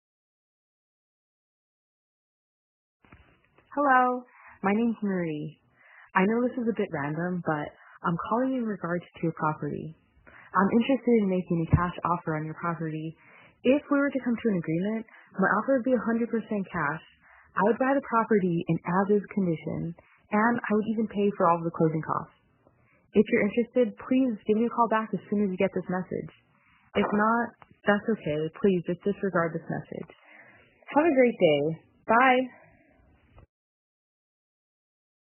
Los Angeles House Flipping Robocall
This is a robocall I received offering to buy "my property" "as is".  Very strange and I did not realize that house flippers in LA were doing this. The phone number was (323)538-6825